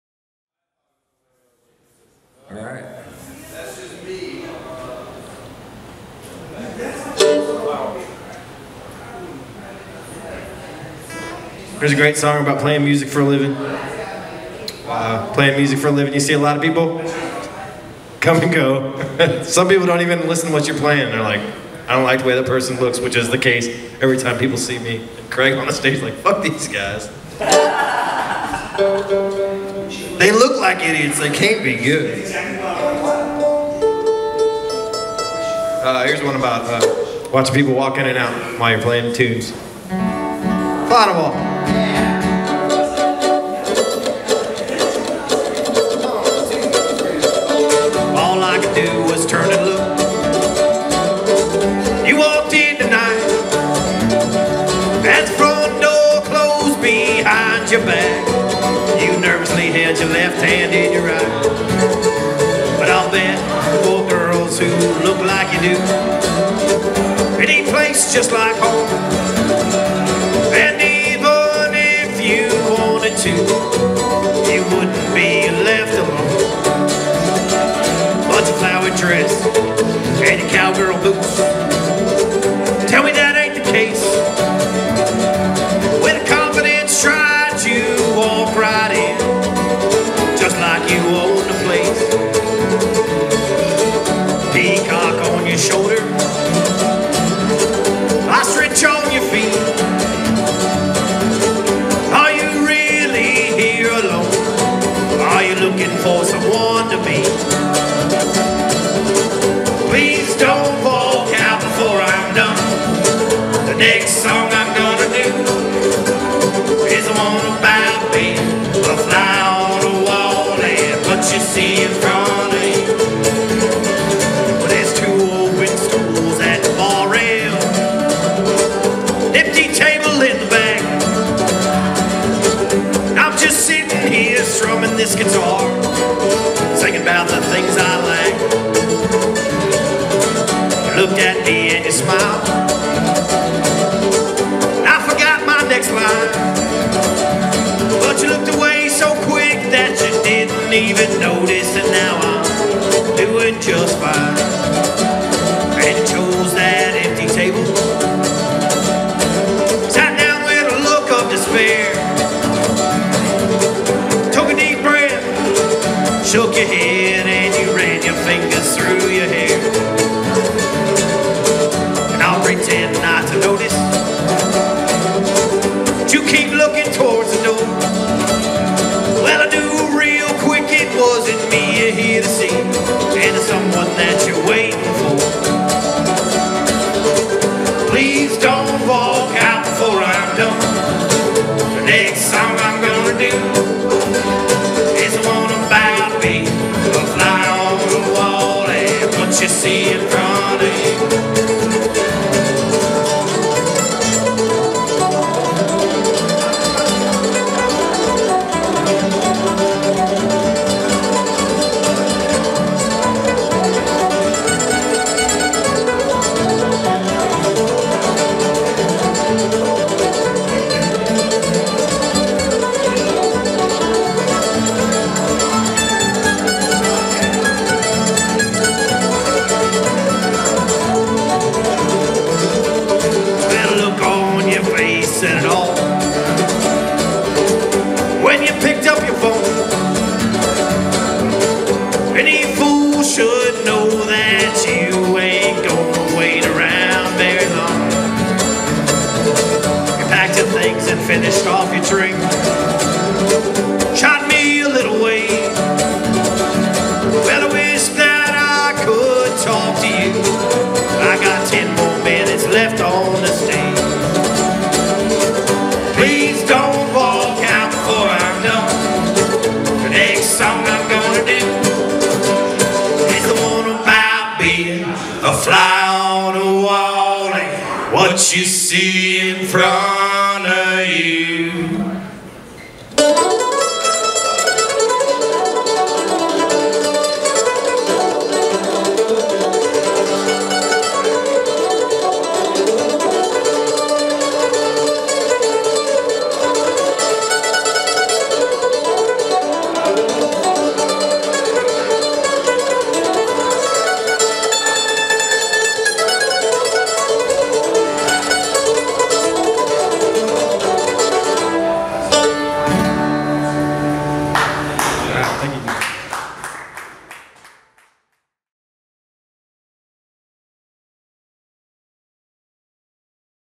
The Shady Rest Band performing at Luna Live
They call their genre Honky Tonk Skiffle Grass. Shady Rest from Austin Texas joined us at Luna Live for an evening of tunes. Here's one of their songs I captured.